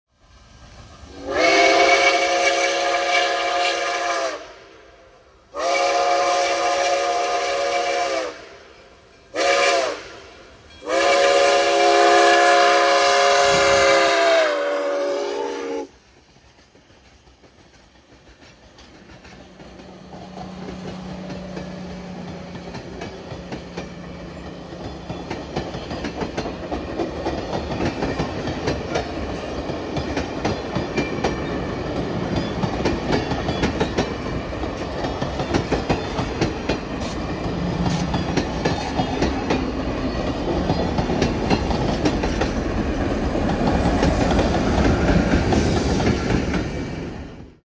August 30th, 2024, Nickel Plate 765 pulls “The Limited” on their way to Reading, Michigan.
August 30th, 2024, Steam locomotive Nickel Plate 765 pulls “The Limited”
through the countryside on their  way to Reading, Michigan as part of
Indiana Rail Experience’s Rolling Victory Weekend.

Where you can find me on the web!